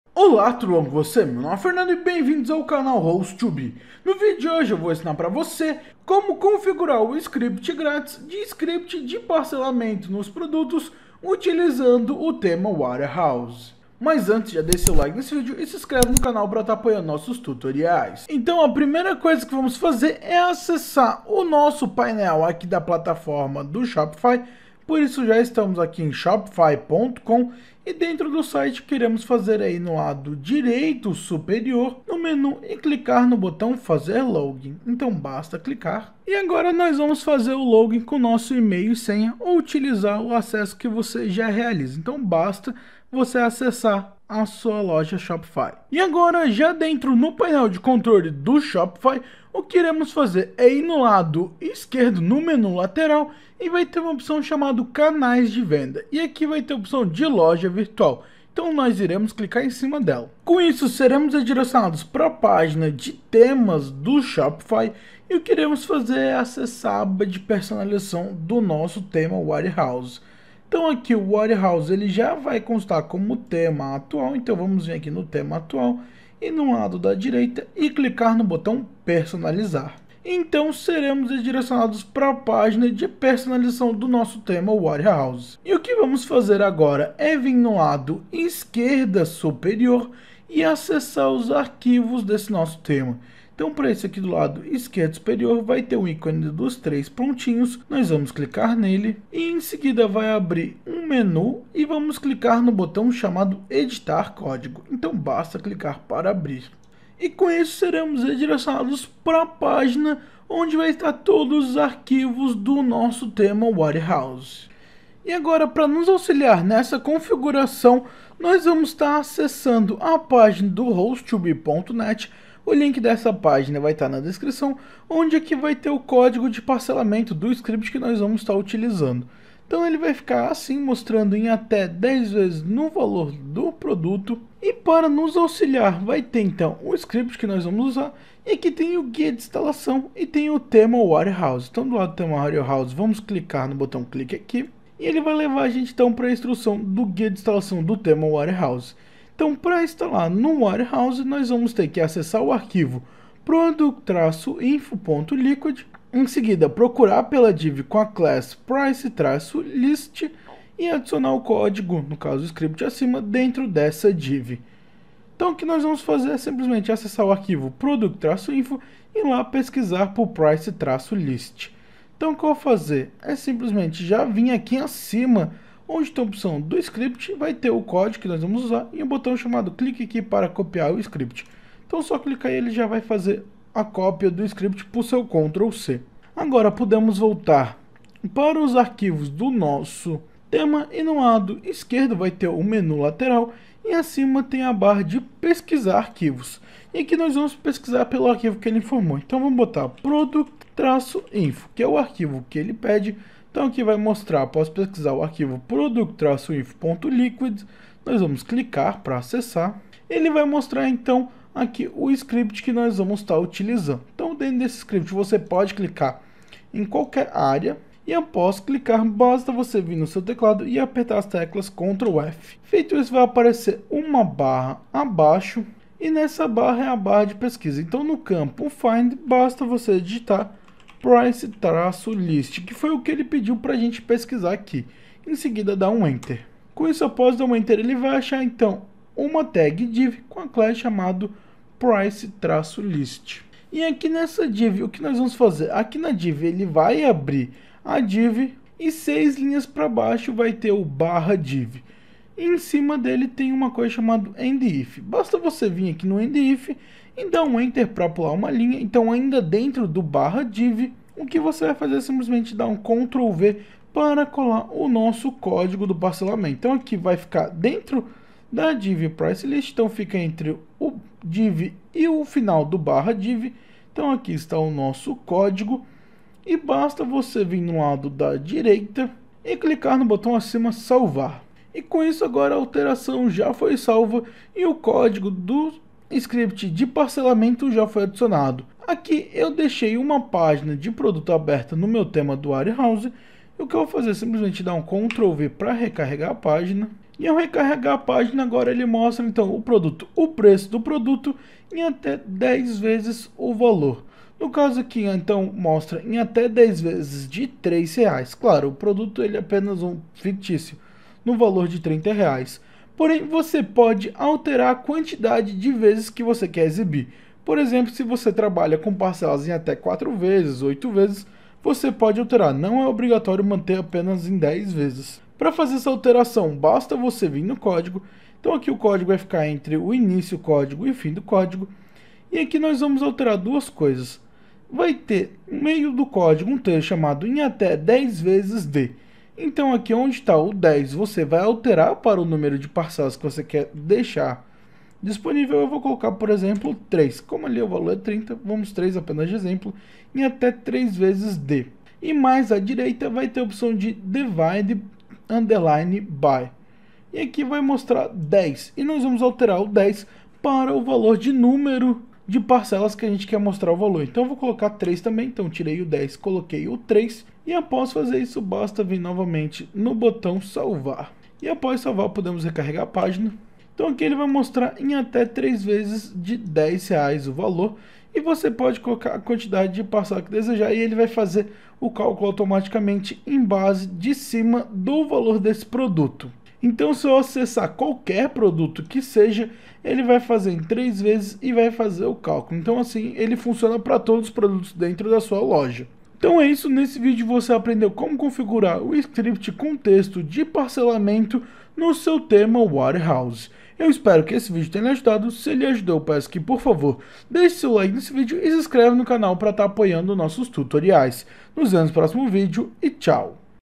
SCRIPT GRATIS: Script de Parcelamento nos Produtos do Warehouse ATUALIZADO 2023
Aprenda como configurar script grátis de parcelamento nos produtos no Warehouse em poucos minutos